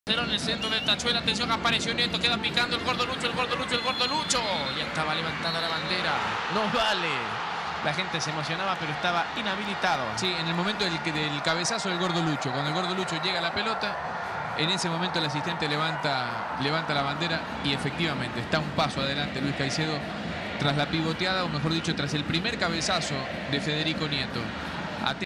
Min 46: Gol anulado a Nieto / BARCELONA - QUITO
Via Claro Sports